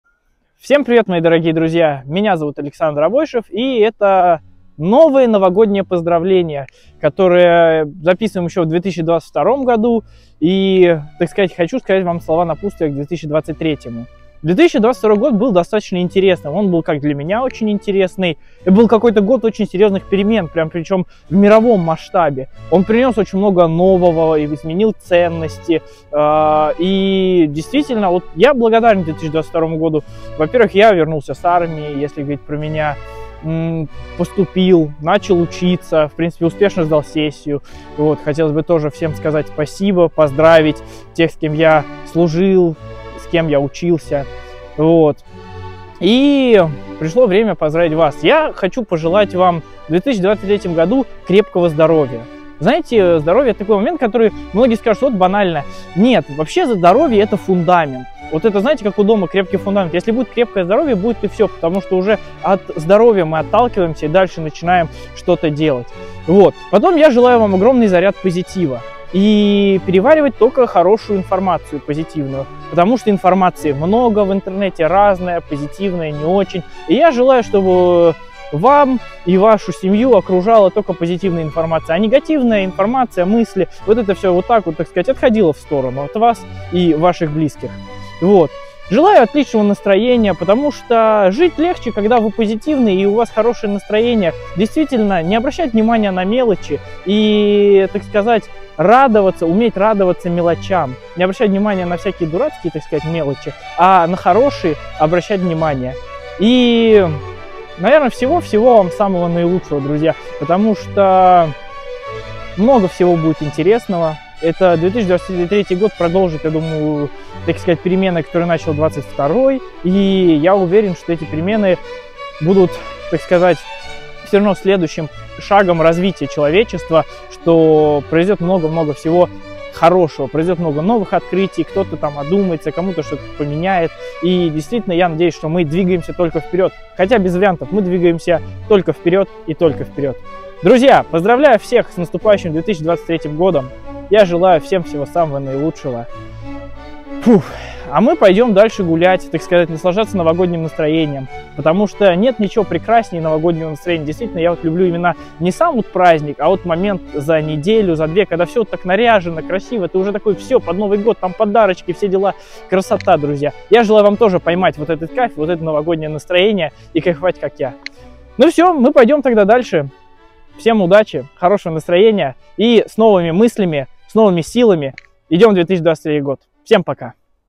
С Наступающим !!!
Друзья, всем большой привет! Сегодня 29 декабря 2022 года, и я хочу вас поздравить с наступающим 2023 годом!